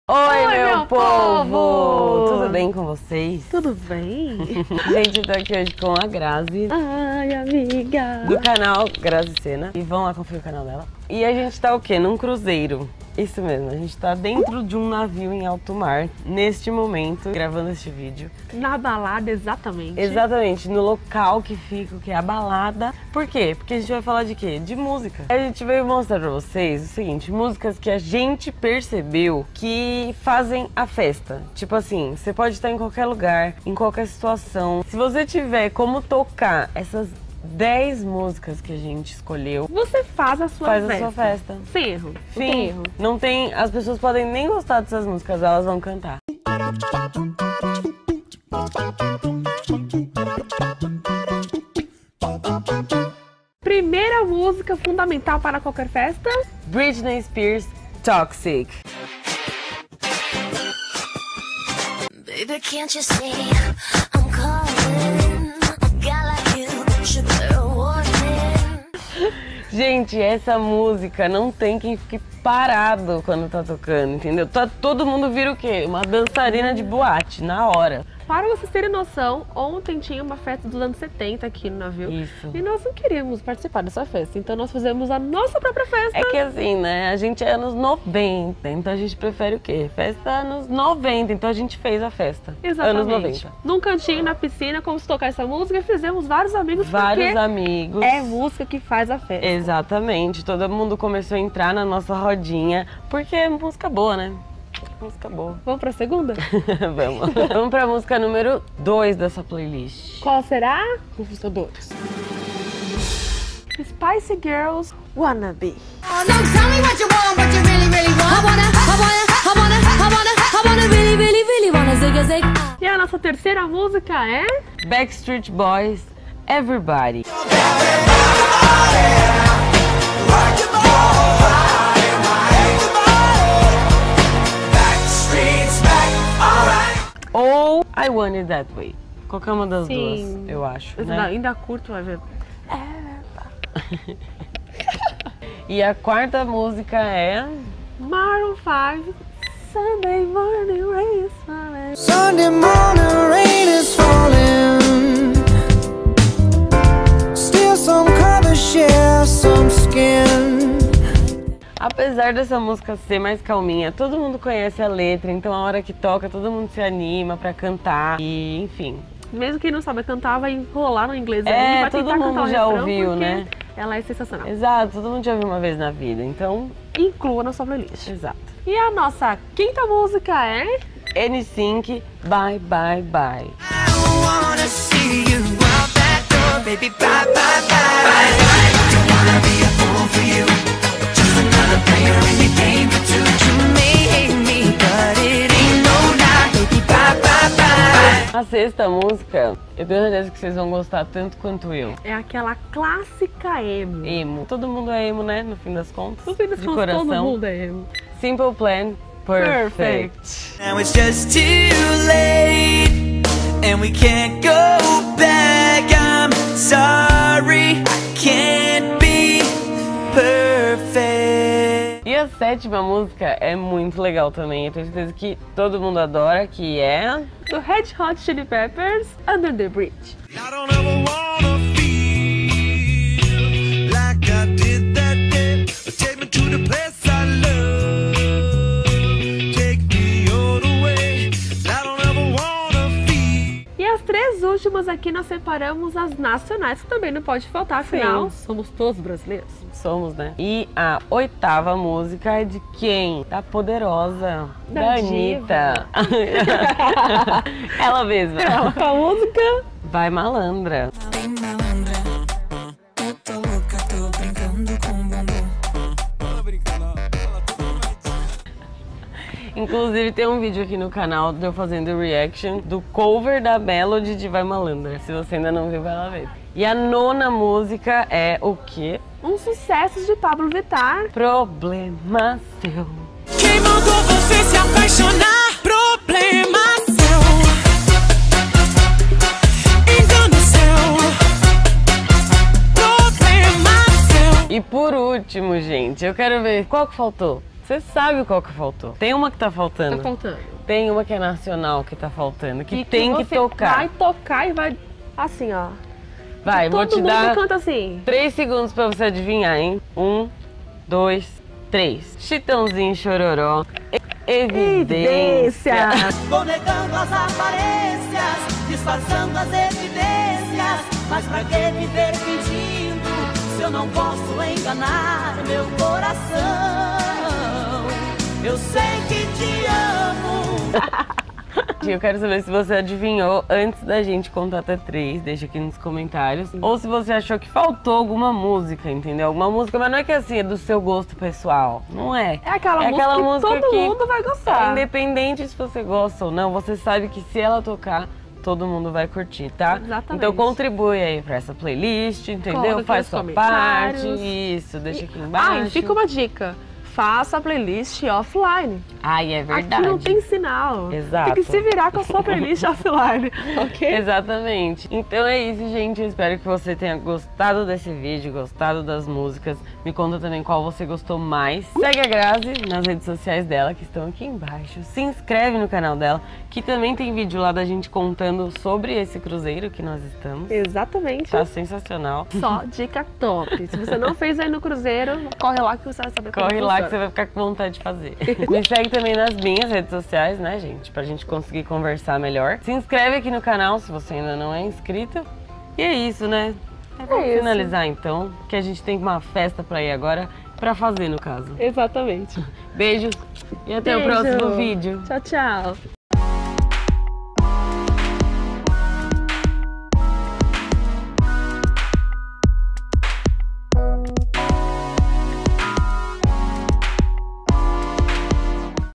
10 MÚSICAS QUE FAZEM A FESTA!!! feat. Grazzi Sena
Genteeeeeeee para tudo!!! Se liga nas 10 músicas que eu e a Grazzi Sena percebemos que FAZEM A FESTA em qualquer lugar!!! Já monta sua playlist, pega o radinho e vai ser feliz! ahahahaha

Sim, em alguns momentos dá para perceber o balanço do navio durante o vídeo, se ficarem com enjoo, me perdoem! ahahahaha

BEIJOS E ATÉ O PRÓXIMO! ⭐🌈🌼